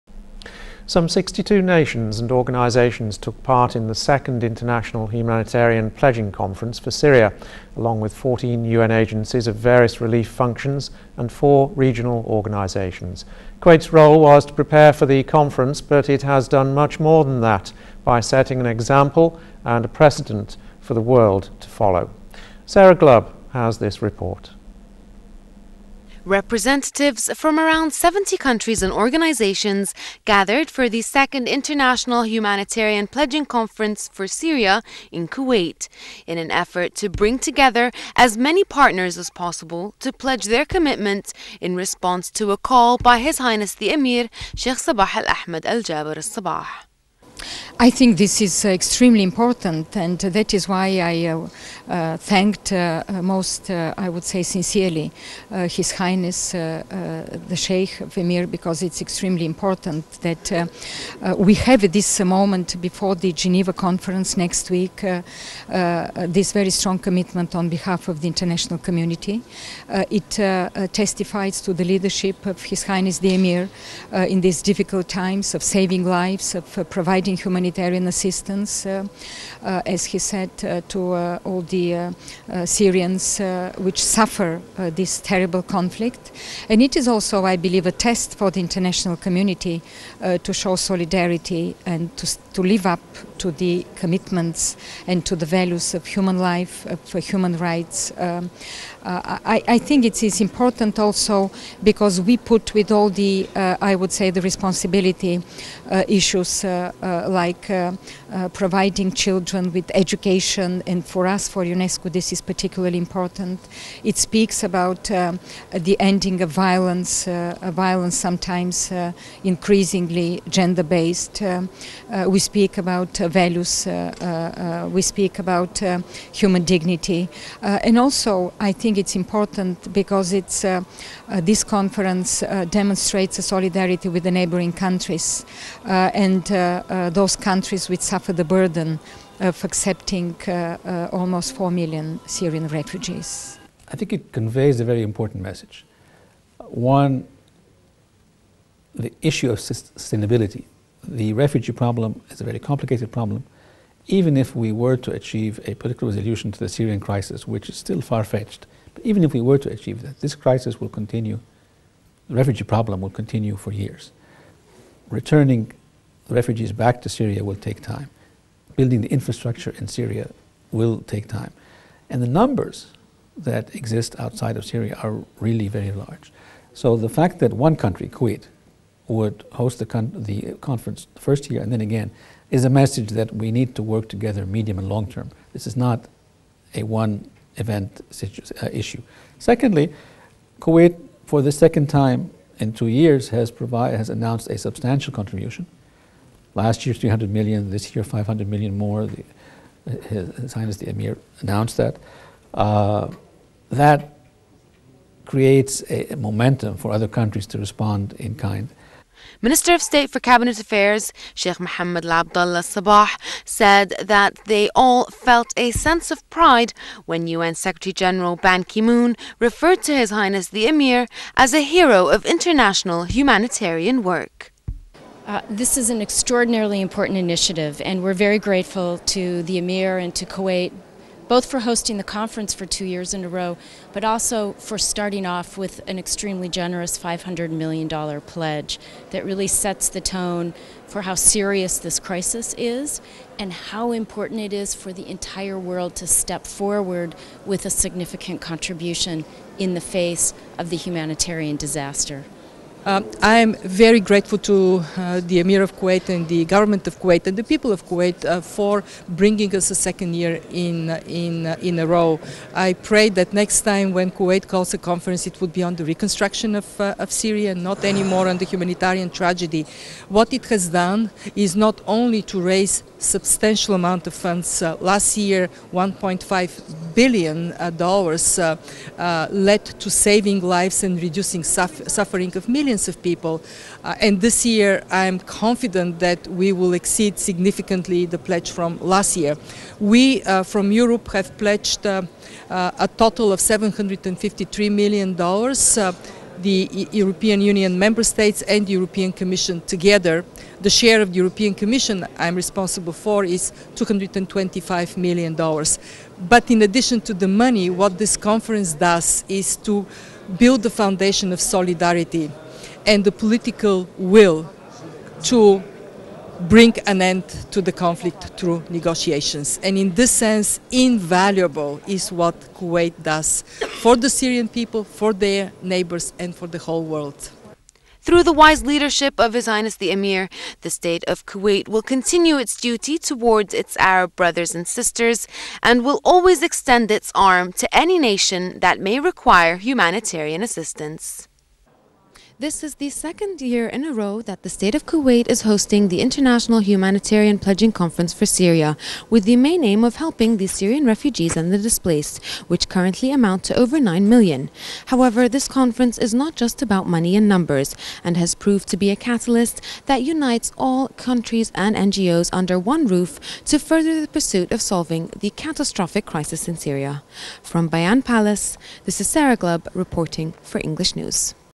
UN Secretary General Ban Ki-moon refers to His Highness the Amir as a "hero of humanitarian work"
Around 70 nations and organizations took part in the Second International Humanitarian Pledging Conference for Syria, along with 14 UN agencies of various relief functions, and four regional organizations.
During the conference, His Highness the Amir Sheikh Sabah Al-Ahmad Al-Jaber Al-Sabah called on citizens, residents, attendees and the international community to exert their efforts towards aiding the Syrian people to alleviate their suffering.
The State of Kuwait's role was to prepare for the conference but it has done much more than that, by setting an example and a precedent for the world to follow.